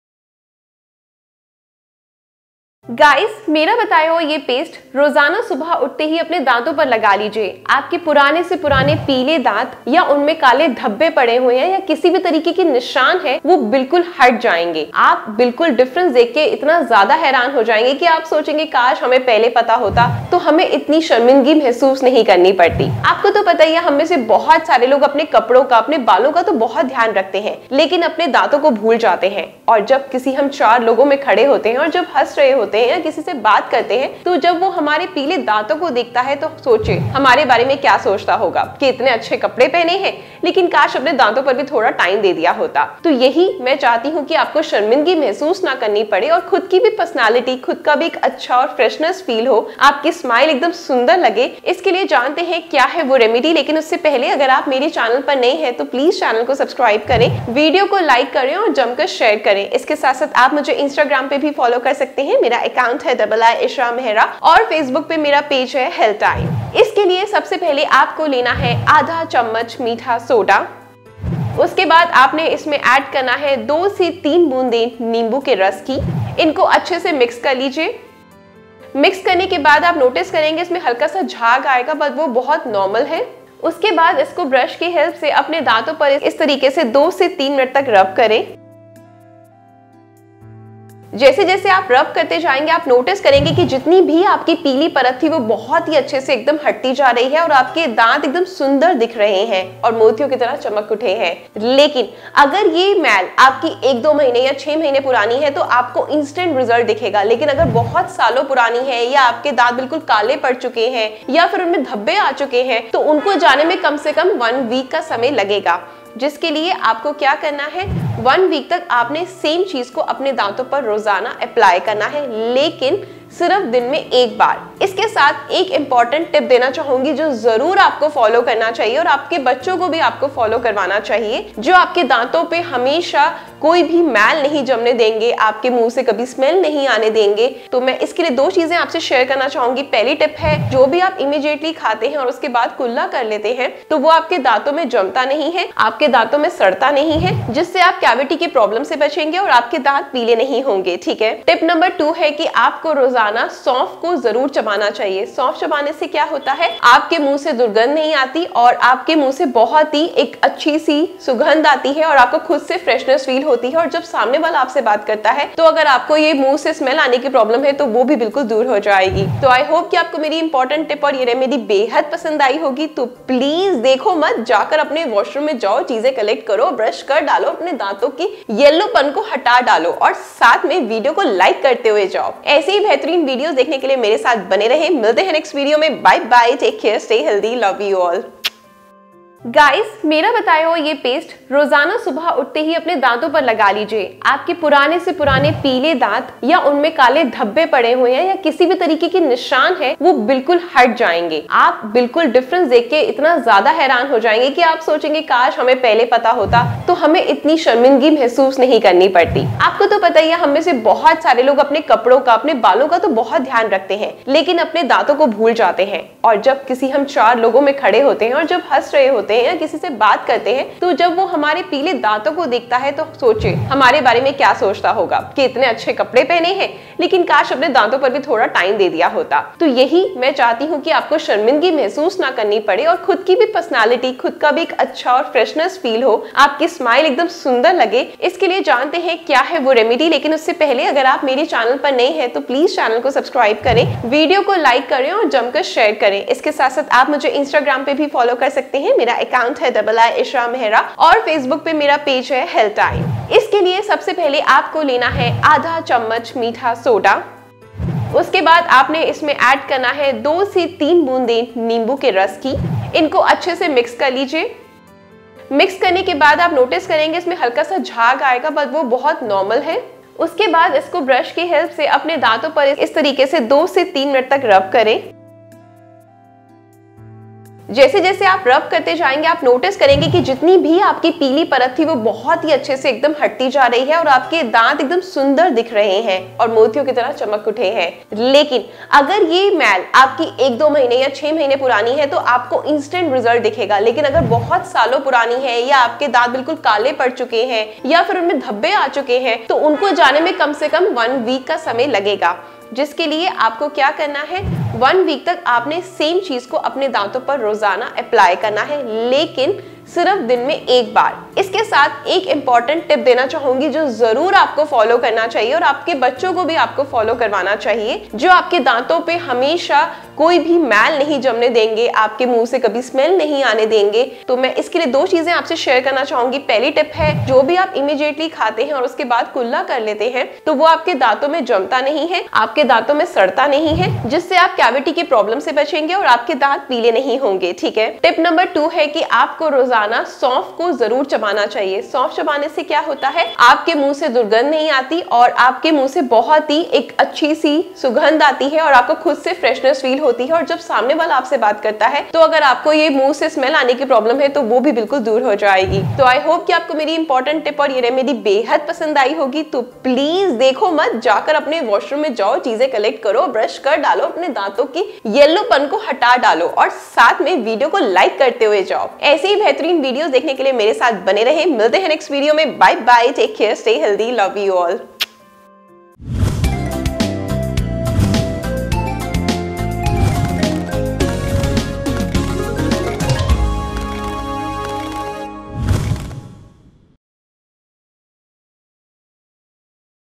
White Teeth home remedy - remove yellowness, remove smell from mouth🙈teethwhitening Dato Ka Pilapan
सिर्फ 2 मिनट में गंदे पीले दाँतो को मोतियों जैसा चमका देगा ये कमाल नुस्खा | teeth whitening at home

 दांतों का पीलापन और बदबू को कैसे दूर करें। free teeth whitening at home